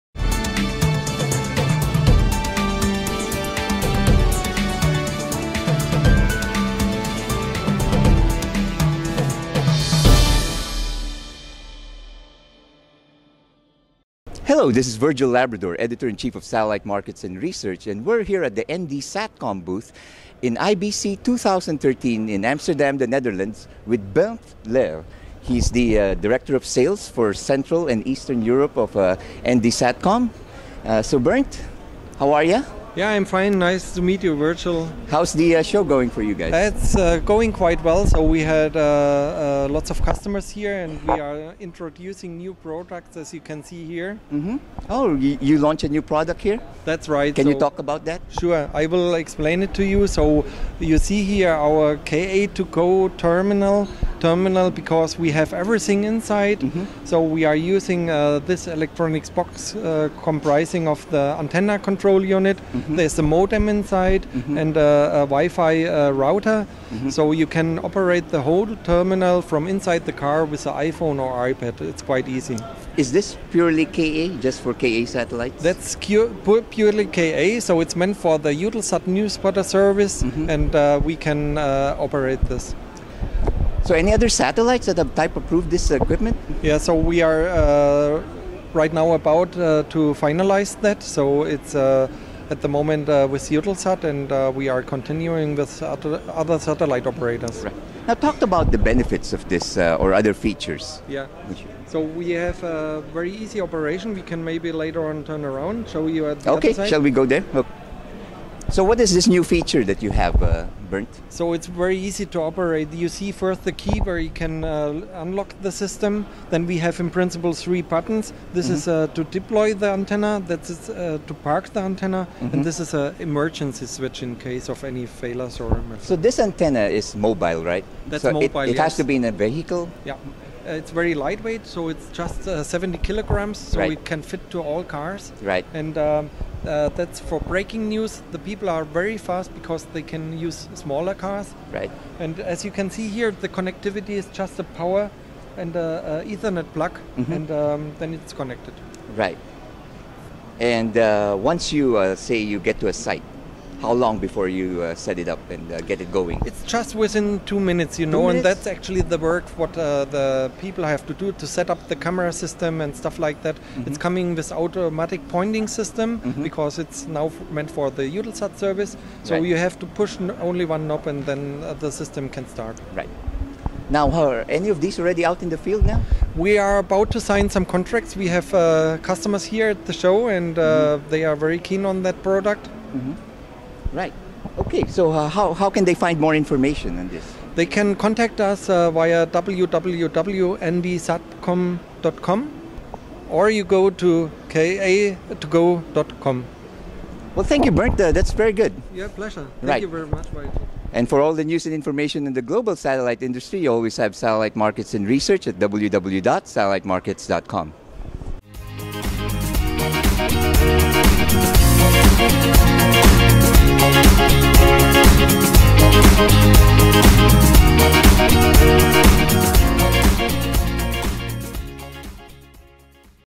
Interview with Bernd Lehr, Sales Director of ND Satcom
At IBC 2013 in Amsterdam, the Netherlands, Satellite Markets and
Research Editor-in-Chief Virgil Labrador spoke with Bernd Lehr, Sales
Director of ND Satcom on the new Ka2Go moible antenna product they
launched at the IBC.